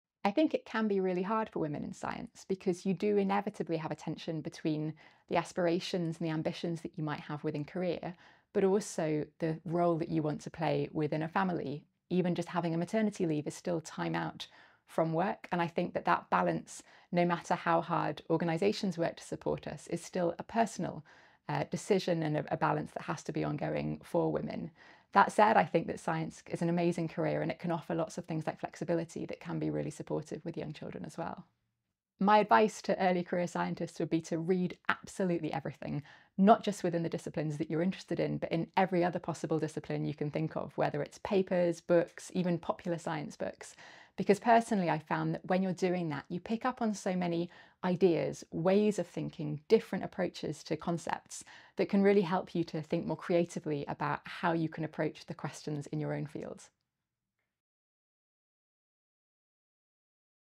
💡Advice for early-career researchers
👩‍🔬 Daisy Fancourt, professor of psychobiology & epidemiology at UCL, reflects on being a woman in science and shares advice for early-career researchers.

Daisy Fancourt’s EXPs here in our collection with The Leverhulme Trust 👉